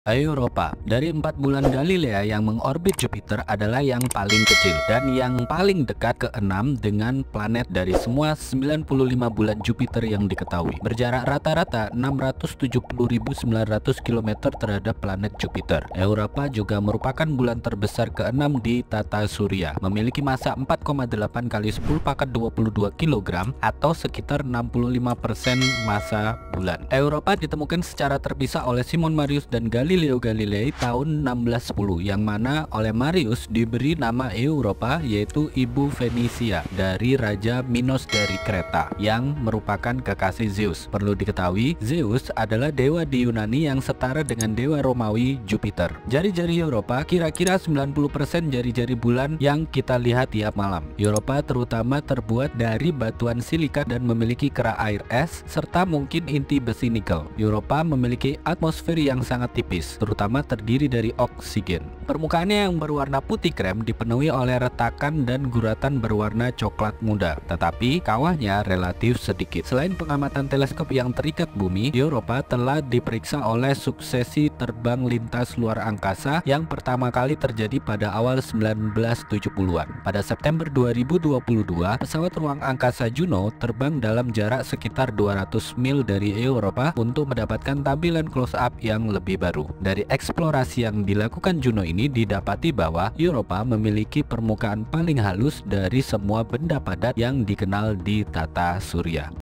PART 4 - Europa[Satelit Jupiter]
Europa atau Jupiter II adalah yang terkecil dari empat bulan Galilea yang mengorbit Jupiter, dan yang terdekat keenam dengan planet dari semua 95 bulan Jupiter yang diketahui.

Berjarak rata - rata 670 900  km terhadap planet Jupiter.

Europa juga merupakan bulan terbesar keenam di Tata Surya memiliki massa 4,8 × 10²²  kg atau sekitar 65 % massa bulan.

Europa ditemukan secara terpisah oleh Simon Marius dan Galileo Galilei tahun 1610 yang mana oleh Marius diberi nama Europa yaitu ibu Fenisia dari Raja Minosdari Kreta yang merupakan kekasih Zeus (dewa di Yunani yang setara dengan dewa Romawi Jupiter).

Jari - jari Europa kira - kira 90% jari - jari Bulan yang kita lihat tiap malam, Europa terutama terbuat dari batuan silikat dan memiliki kerak air-es serta mungkin inti besi-nikel.

Europa memiliki atmosfer yang sangat tipis, terutama terdiri dari oksigen.

Permukaannya yang berwarna putih krem ​​dipenuhi oleh retakan dan guratan berwarna cokelat muda, tetapi kawahnya relatif sedikit.

Selain pengamatan teleskop yang terikat Bumi, Europa telah diperiksa oleh suksesi terbang lintas luar angkasa, yang pertama kali terjadi pada awal 1970-an.

Pada September 2022, pesawat ruang angkasa Juno terbang dalam jarak sekitar 200 mil dari Europa untuk mendapatkan tampilan close-up yang lebih baru.

Dari eksplorasi yang dilakukan Juno ini didapati bahwa Europa memiliki permukaan paling halus dari semua benda padat yang dikenal di Tata Surya.

Kemudaan dan kehalusan permukaan yang tampak telah mengarah pada hipotesis bahwa lautan air ada di bawah permukaan, yang dapat menampung kehidupan di luar bumi.

Model dominan menunjukkan bahwa panas dari pelenturan pasang surut menyebabkan lautan tetap cair dan mendorong pergerakan es yang mirip dengan lempeng tektonik, menyerap bahan kimia dari permukaan ke laut di bawahnya.

Garam laut dari samudra di bawah permukaan mungkin melapisi beberapa fitur geologis di Europa, menunjukkan bahwa samudra berinteraksi dengan dasar laut.

Ini mungkin penting dalam menentukan apakah Europa dapat dihuni.

Selain itu, Teleskop Luar Angkasa Hubble mendeteksi semburan uap air mirip dengan yang diamati di bulan Saturnus Enceladus, yang diperkirakan disebabkan oleh cryogeysers yang meletus.

Pada Mei 2018, para astronom memberikan bukti pendukung aktivitas semburan air di Europa, berdasarkan analisis terbaru dari data yang diperoleh dari Galileo pesawat luar angkasa, yang mengorbit Jupiter dari tahun 1995 hingga 2003.

Aktivitas membanggakan seperti itu dapat membantu para peneliti dalam mencari kehidupan dari bawah permukaan samudra Eropa tanpa harus mendarat di bulan.

Misi Galileo , diluncurkan pada tahun 1989, menyediakan sebagian besar data terkini di Europa.

Belum ada pesawat ruang angkasa yang mendarat di Europa, meskipun ada beberapa misi eksplorasi yang diusulkan.

Jupiter Icy Moon Explorer (JUICE) Badan Antariksa Eropa adalah misi ke Ganymede yang diluncurkan pada 14 April 2023 yang akan mencakup dua terbang lintas Europa.

Europa Clipper NASA diperkirakan akan diluncurkan pada Oktober 2024, dengan kemungkinan pendarat pelengkap berdasarkan temuannya.